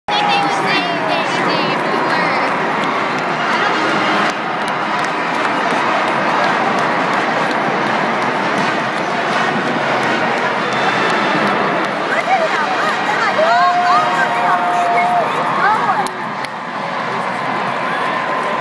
Touchdown SFA
Cheerleaders celebrate a touchdown at the Reliant Stadium